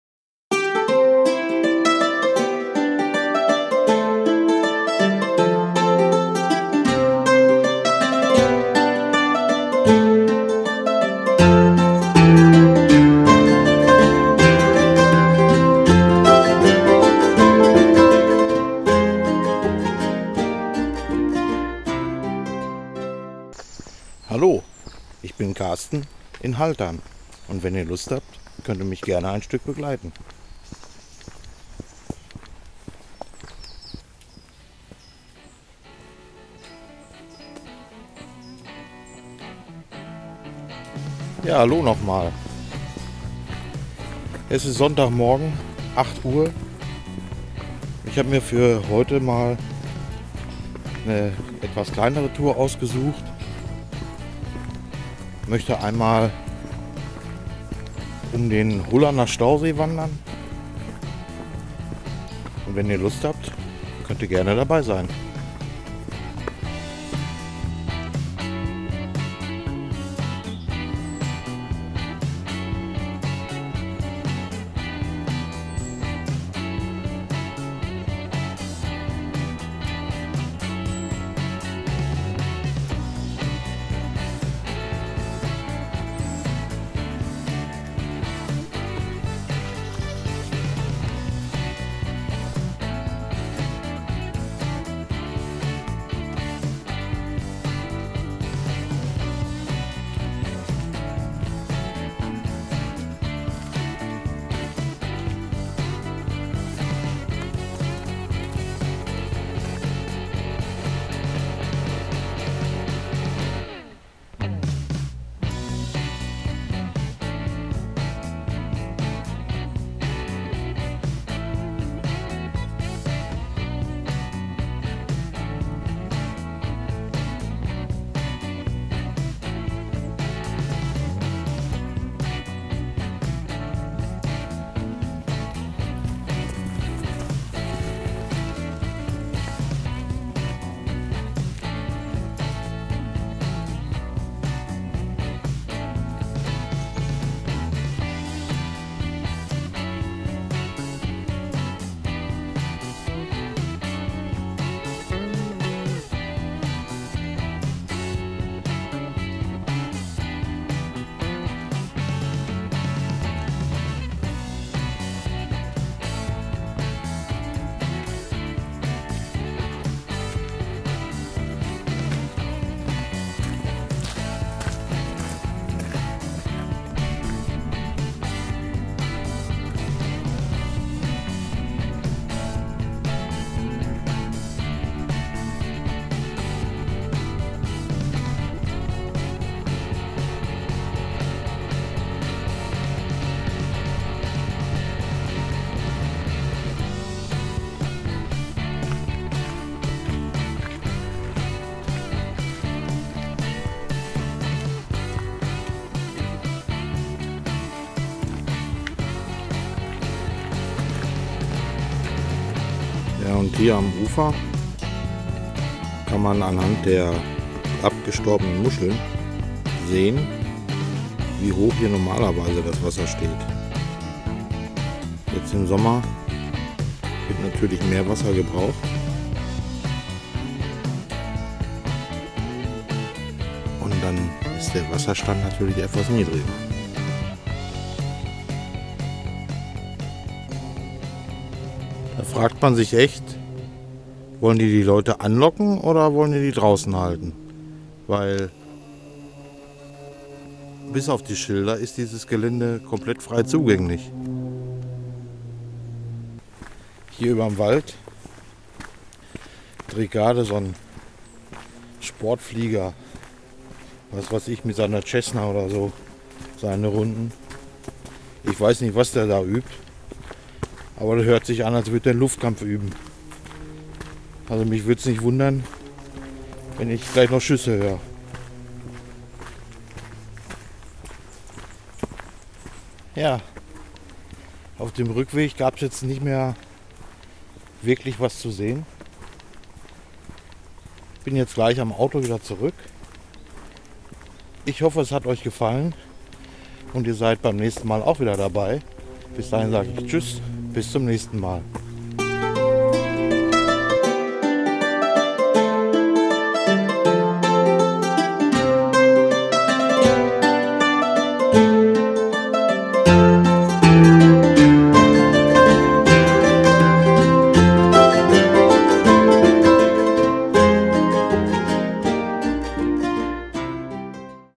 Nach dem Sturm am Hullerner Stausee
Nachdem Samstags sturmbedingt kein Wanderwetter war ging es am Sonntag um den Hullerner Stausee.